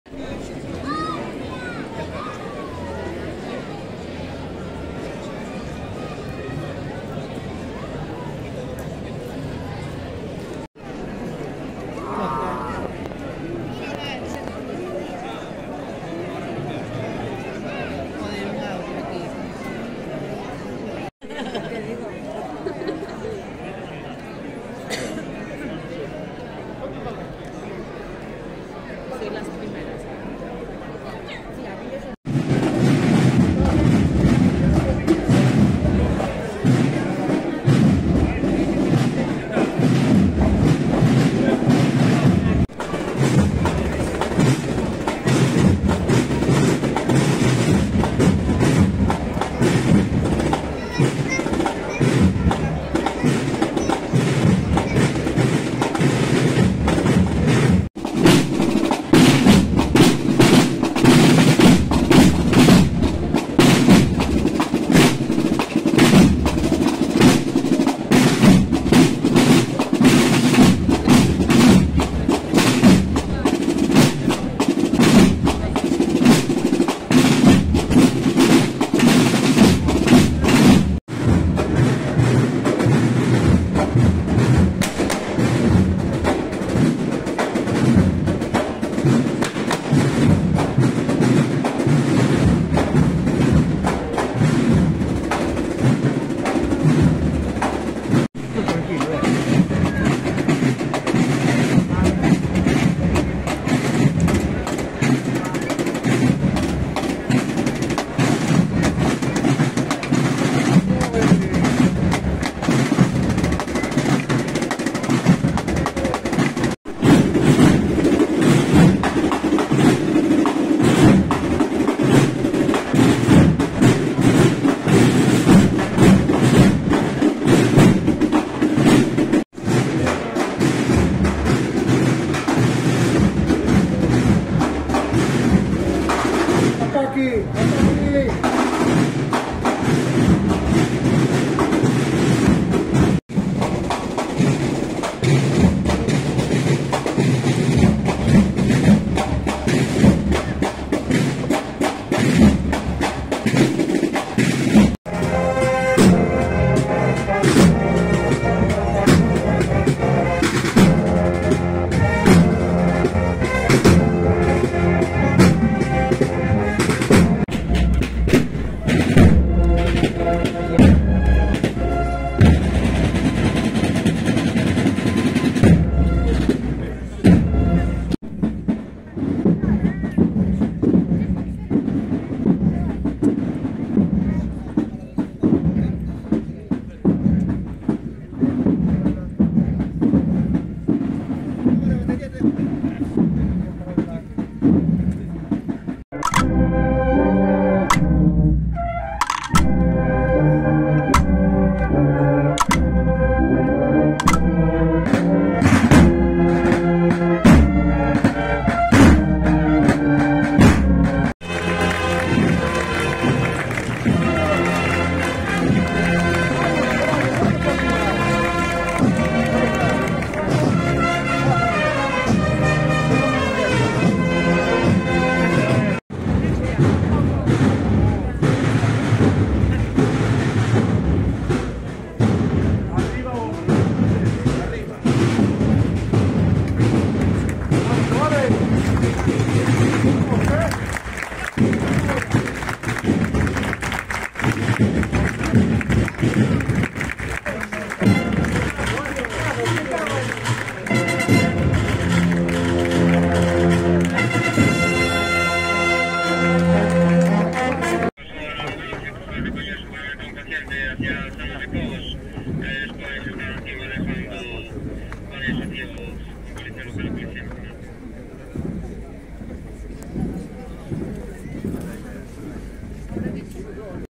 El Prendimiento y la Misericordia recorren las calles de Viveiro
Imágenes de las procesiones de El Prendimiento y de la Misericordia durante la Semana Santa de Viveiro, en Lugo.

(Fuente: EP)